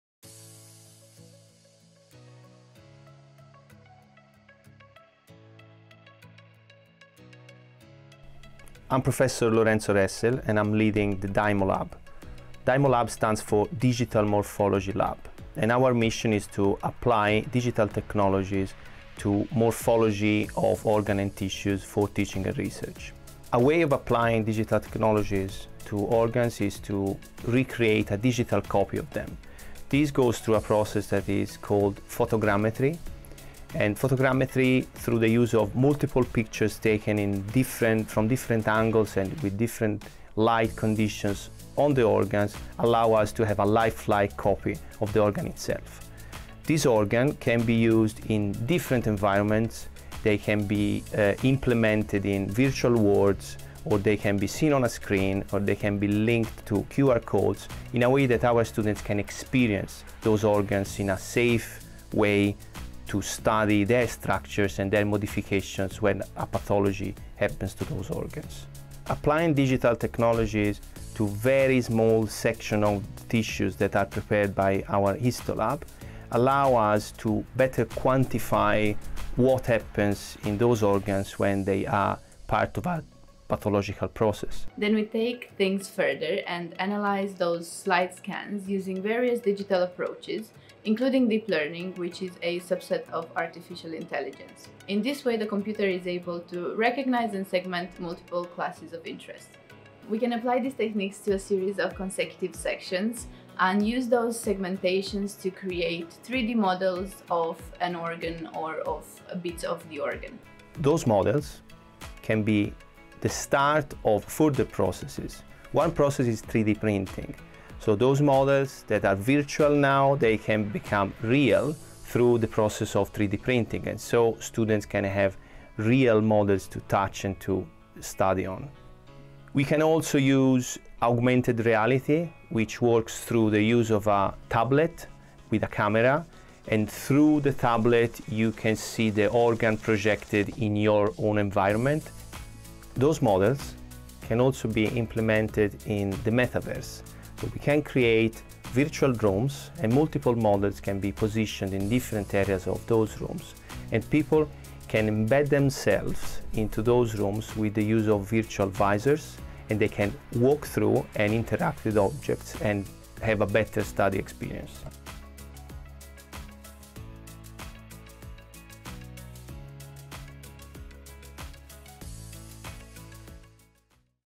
In The Lab - DiMo Lab
Based within our School of Veterinary Science, the DiMo (Digital Morphology) Lab is a fully equipped state-of-the-art facility for teaching and research.

Led by Professor Lorenzo Ressel, the lab uses advanced technology like scanning, image analysis, deep learning, and 3D printing to make research and diagnostics easier and more accurate. AI helps improve data consistency and automates tasks.

Credits. NVIDIA®: GPU support; RealityCapture®: photogrammetry software; Museum container room: Sketchfab “Boathouse” by dylanheyes.